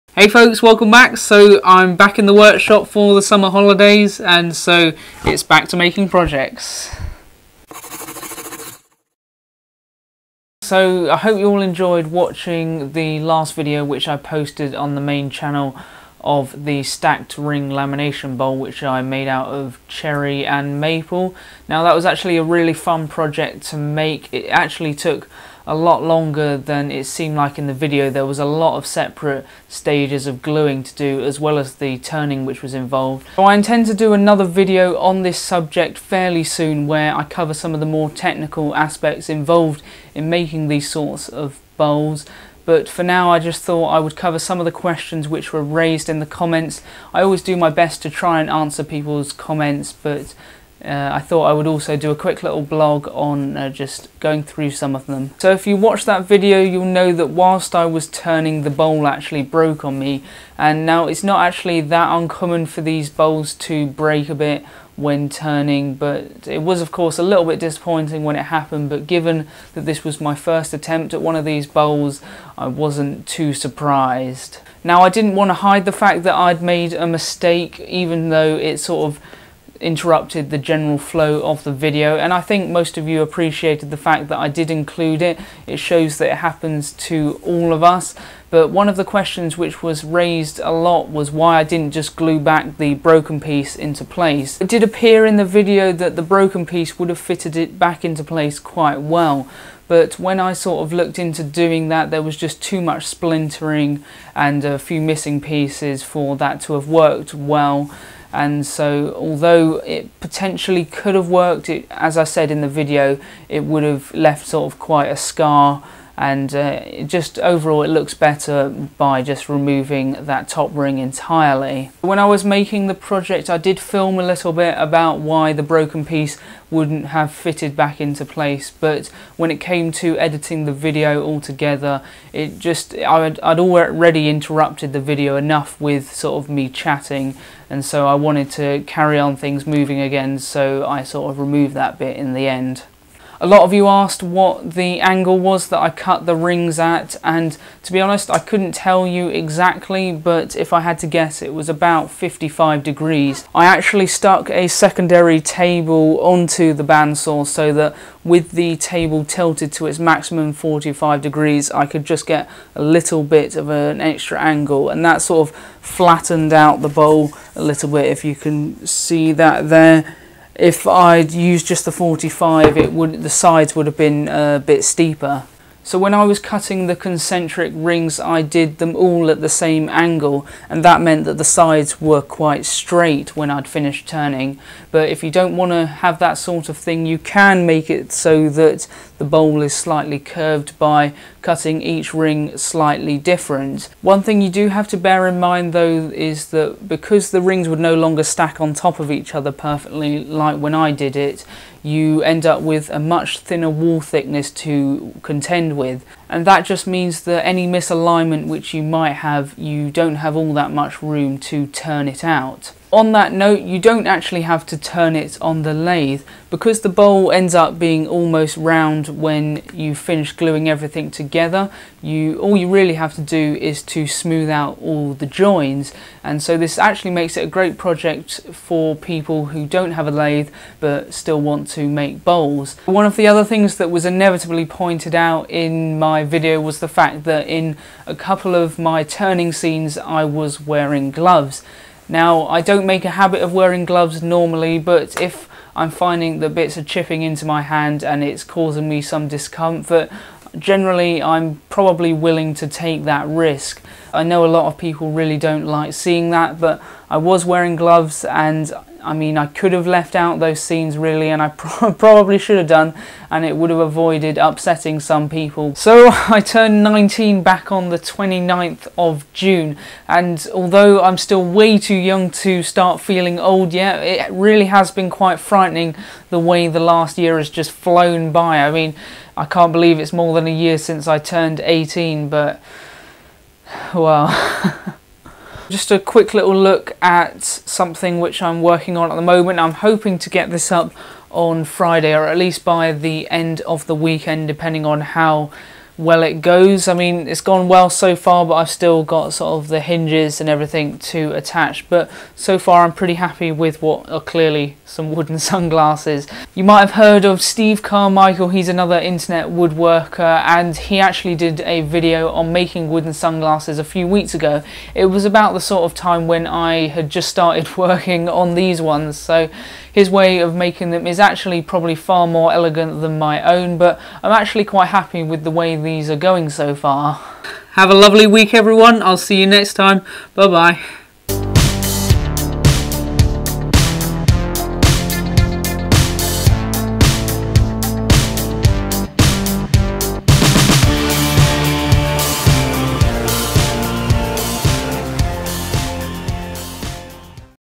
Stacked Ring Bowl Follow-up and Sunglasses Shocker
Answering some of the questions from my recent project video "The Big Bowl Turning: Stacked Ring Technique"
There's also a little project preview for you, watch until the end of the video, if you dare!